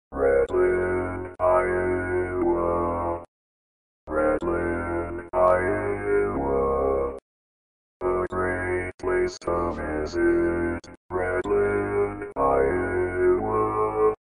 How to Say or Pronounce USA Cities — Readlyn, Iowa
This video shows you how to say or pronounce Readlyn, Iowa.

A computer said Readlyn, Iowa. How would you say Readlyn, Iowa?